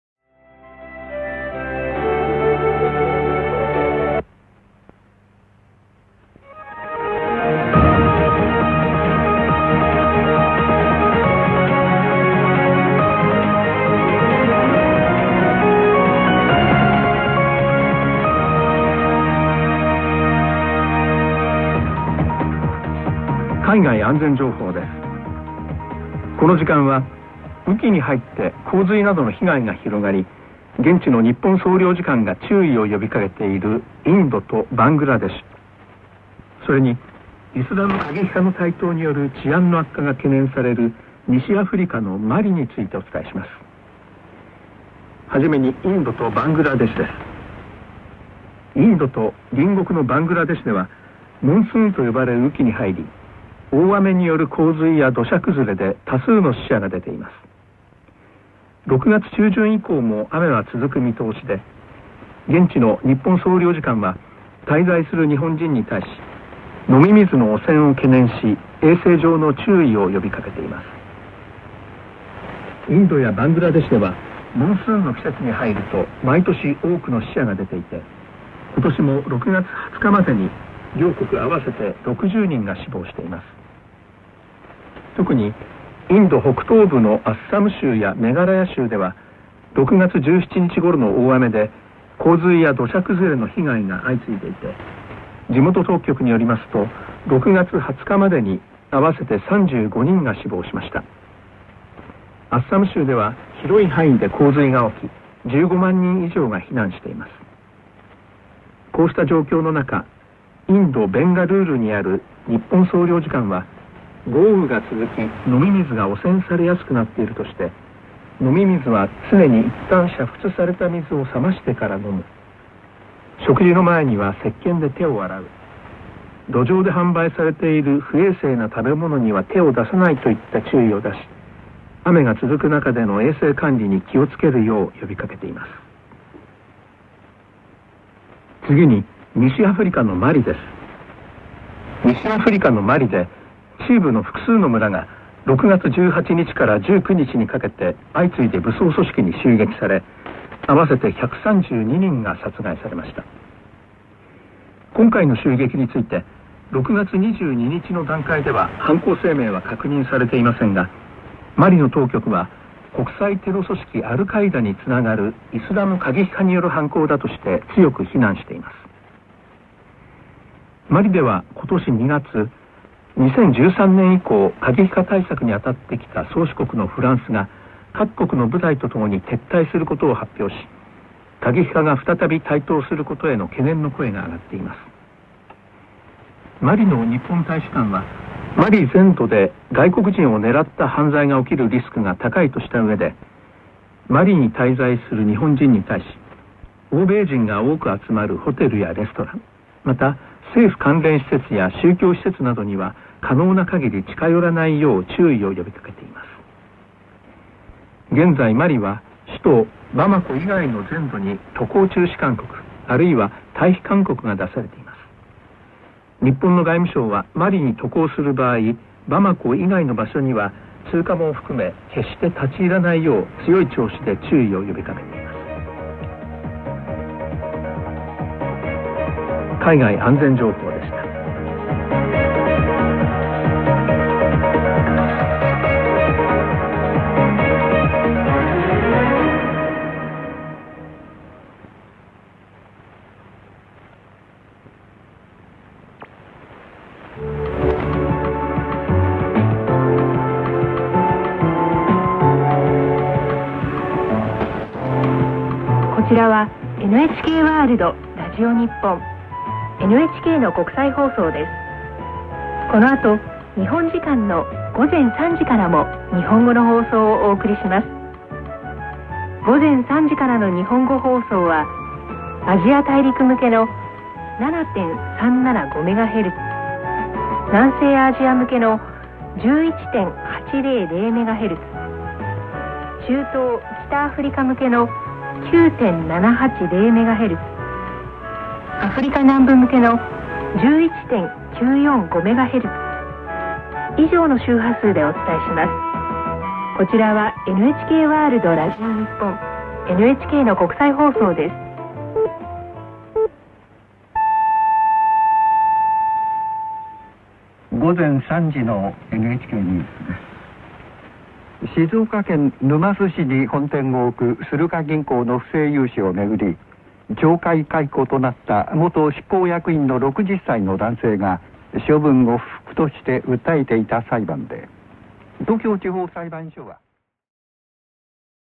NHK World Radio Japan: radio news in Japanese language. 23 Jun 2022
NHK  World Radio Japan on 9780 khz AM [17:00 UTC-19:00 UTC slot]

0:00 Radio news
4:06 Frequencies schedules and technical info
5:01 Time signal

Programs broadcasted from Issoudun in France
Received with an XHDATA D-808 in northern Italy